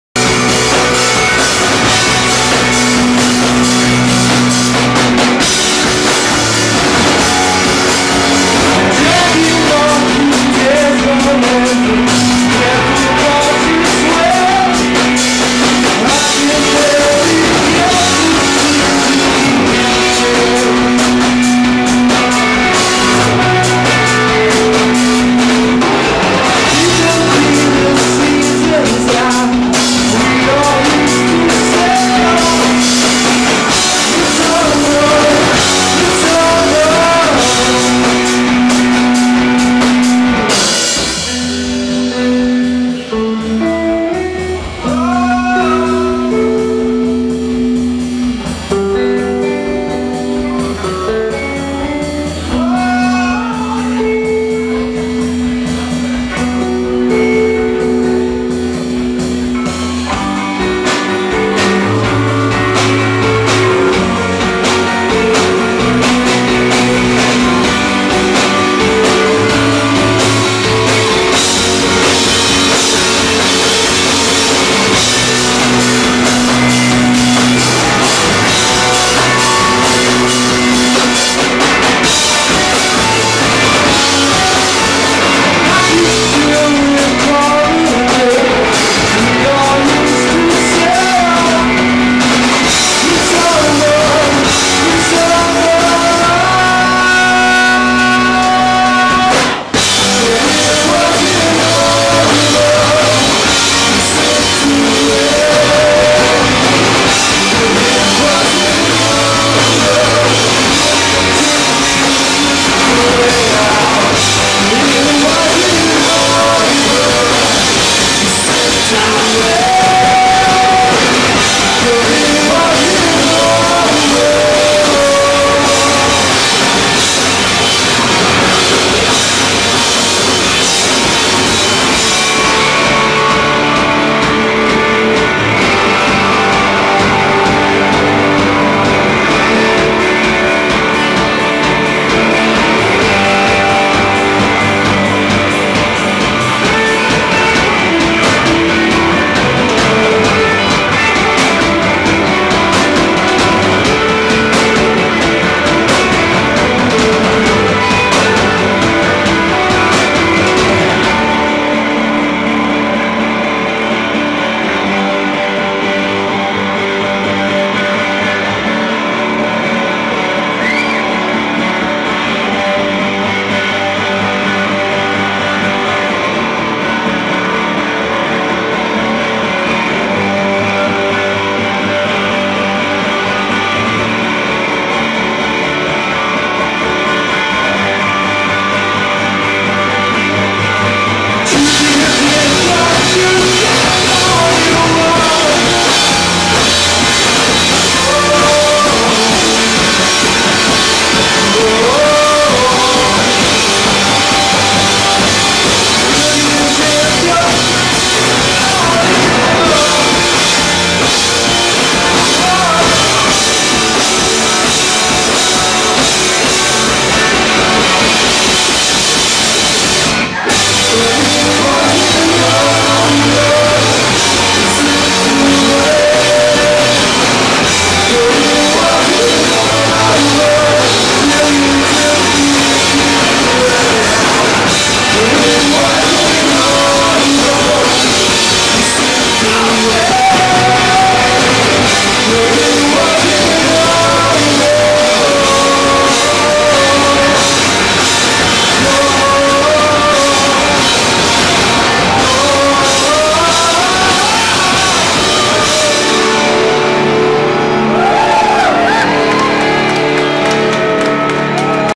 Dreamtigers
Dreamtigers live at Middle East Upstairs 8/3/2011